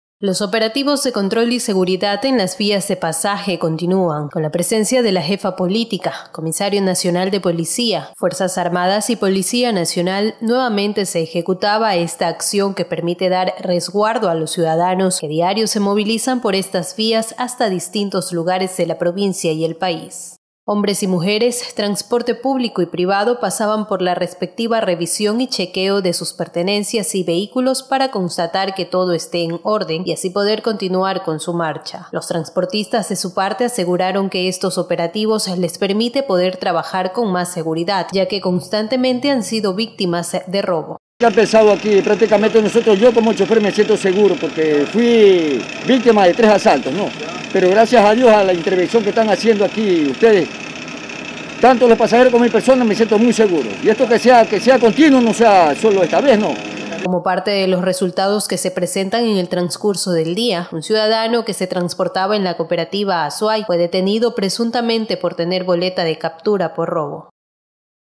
📍PASAJE|| CONTINÚAN OPERATIVOS DE CONTROL Y SEGURIDAD EN VÍAS
Con la presencia de autoridades, Fuerzas Armadas y Policía Nacional, nuevamente se ejecutaban operativos que permiten dar resguardo a ciudadanos que a diario se movilizan por las vías de Pasaje hasta distintos lugares de la provincia y el país.